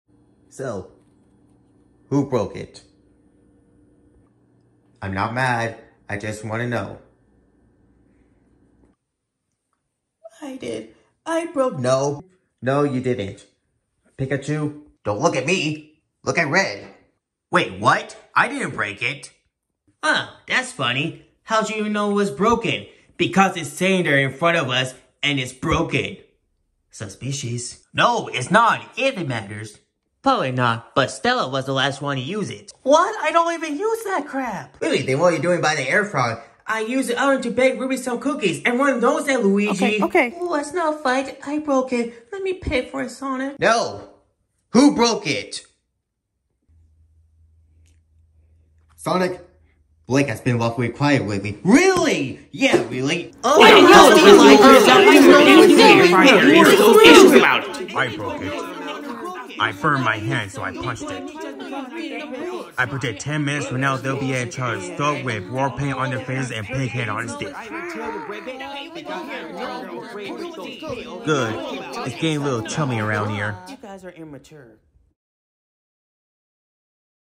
Plush Short: Who broke it
Plush short video on whoever did broke the air fryer. Who broke it? Find out now.
My air fryer was broken 2 days ago while I'm making some burgers, might have to get the new one. But whenever I seen this kind of video like this This one gives me a great idea for the plush short. Hope you enjoy.

Credit to Lemon_Eyebrow for this Knuckles art. Thanks for the permission!